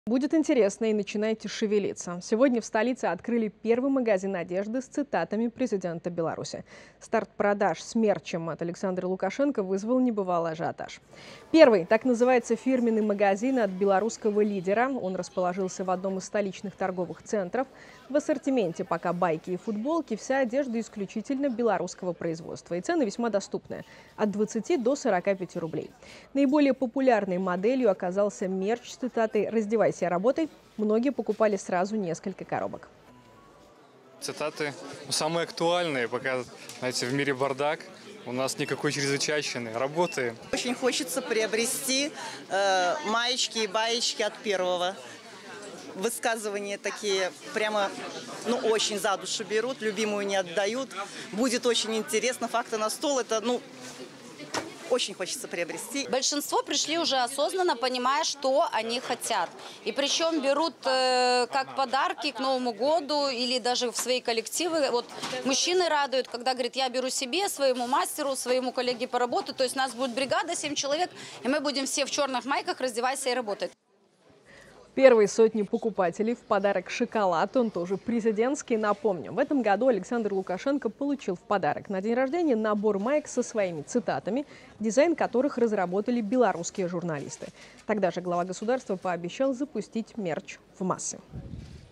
«Раздевайся и работай»: в Минске открылся магазином мерча с цитатами Лукашенко
«Будет интересно» и «начинайте шевелиться». В Минске открыли магазин мерча «Первый» с цитатами Александра Лукашенко.